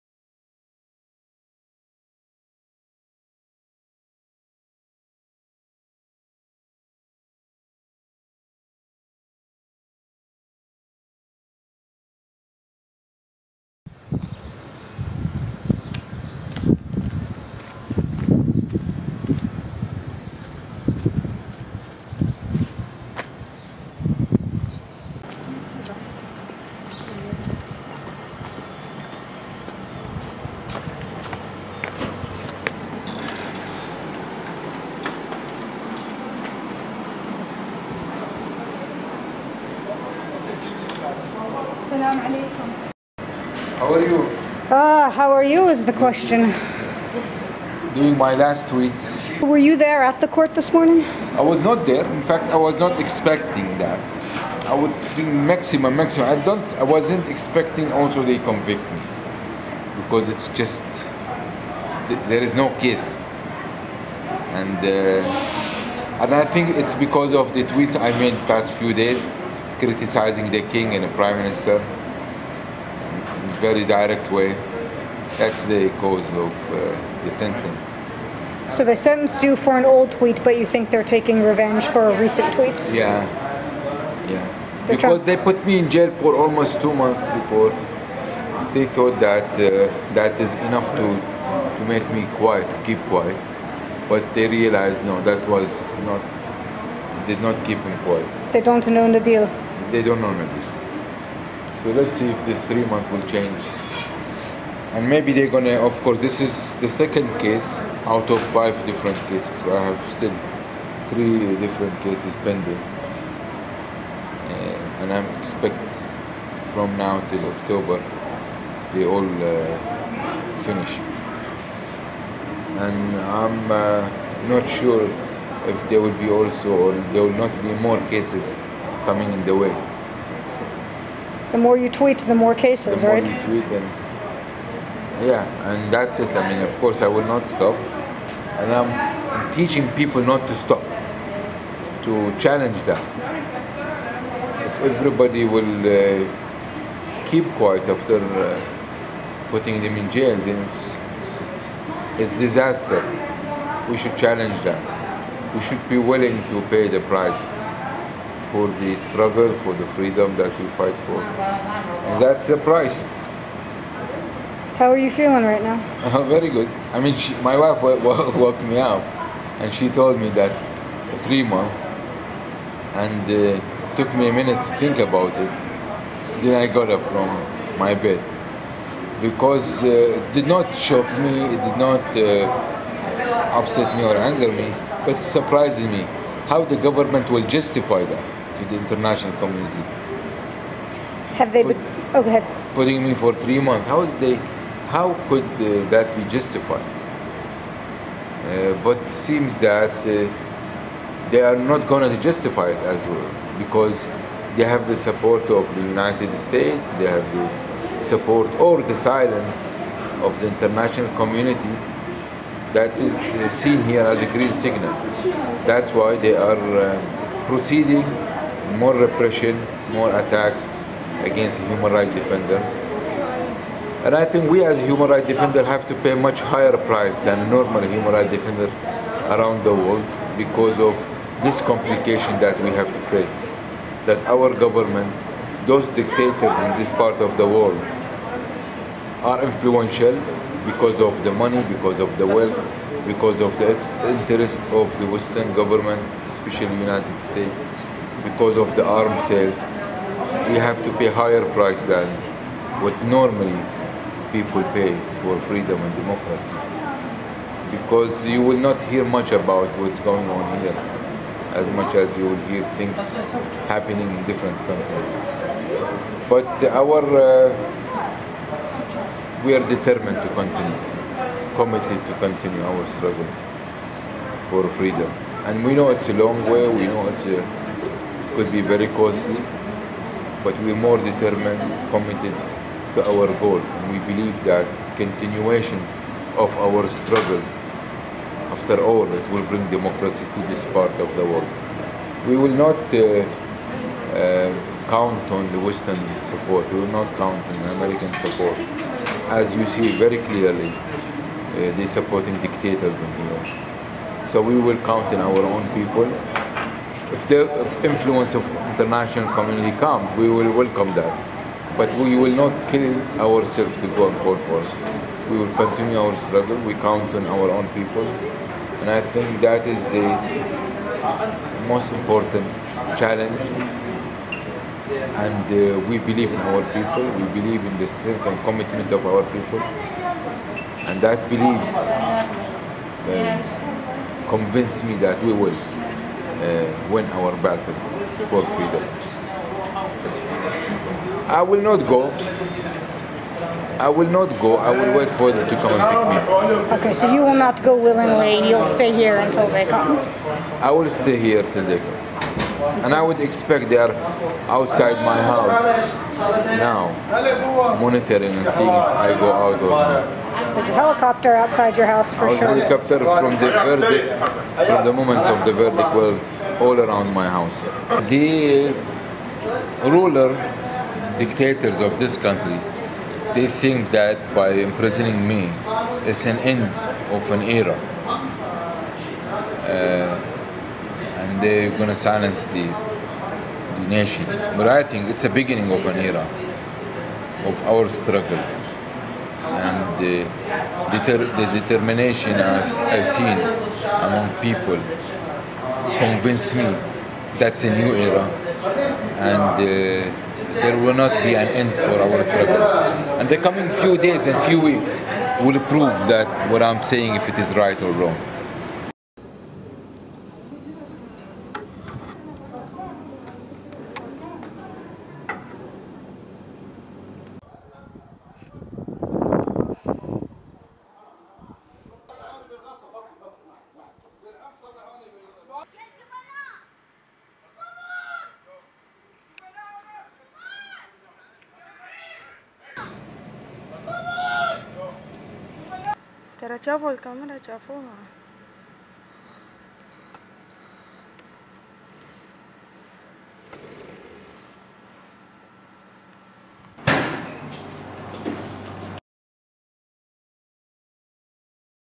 Nabeel Rajab Arrested in Bahrain
9 July 2012 - Human rights defender Nabeel Rajab was arrested from his home after being sentenced to 3 months in jail for sending a tweet that was critical of the Bahraini Prime Minister.  Before being arrested, Rajab retweeted the same tweet that led to the case against him.